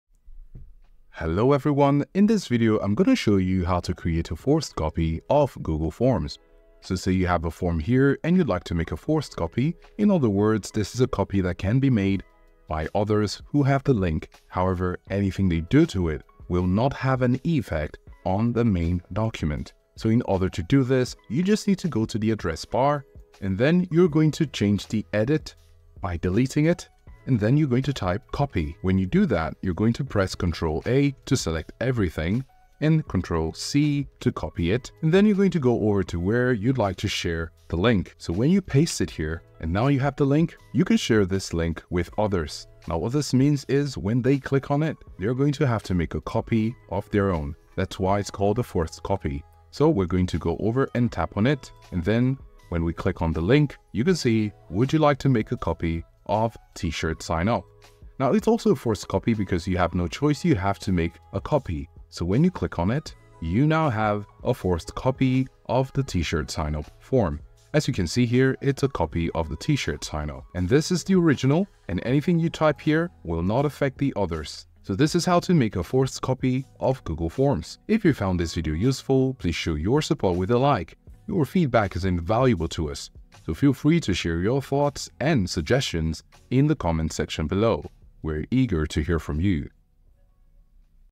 How to Create a Forced Copy of Google Forms (Easy Tutorial)
Welcome to our tutorial on How to Create a Forced Copy of Google Forms! In this video, you'll learn the step-by-step process to ensure others can make their own copies of your Google Form without altering the original. Follow along to master this useful technique!

Timestamps:

00:01 Introduction to creating a forced copy of Google Forms
00:14 Modifying the URL from "edit" to "copy"
00:29 Copying and sharing the modified URL
00:44 Recipients' process of making their own copy
00:58 Final results and confirmation

What you will learn:

How to modify the Google Forms URL to create a forced copy.
The steps to share a link that ensures others make their own copy of the form.
How this method keeps your original Google Form untouched while allowing others to use it.